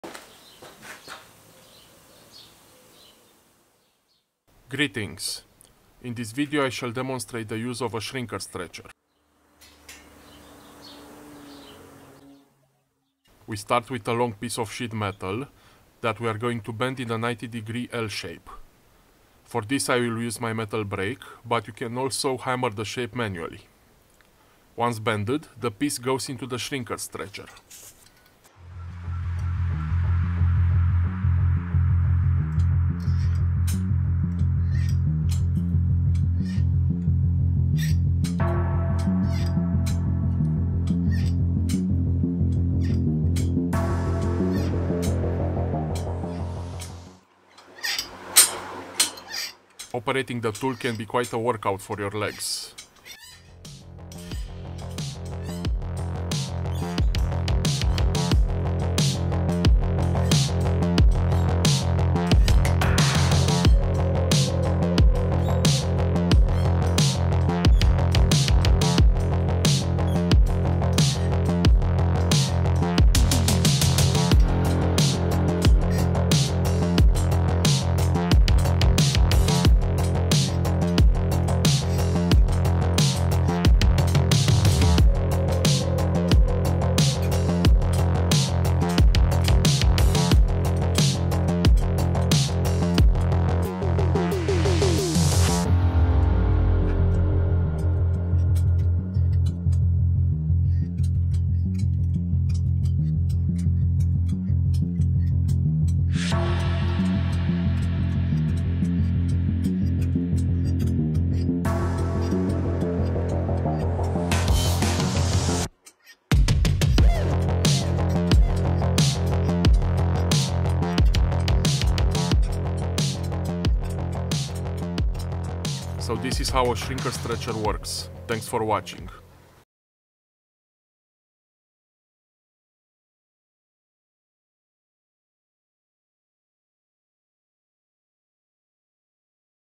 Demonstrating the metal shrinker-stretcher. A must have tool in the D.I.Y. workshop!
The shrinker stretcher does what its name implies: it shrinks or stretches a metal lip allowing the creation of various rounded metal lips which in turn can be used to create replacement panels for vintage car metal work, for metal art projects or various metal fabrication elements. Today I am going to demonstrate its use with brass, but of course, it can be used with all the usual metals found in the DIY shop: steel, aluminum, copper,  and so on.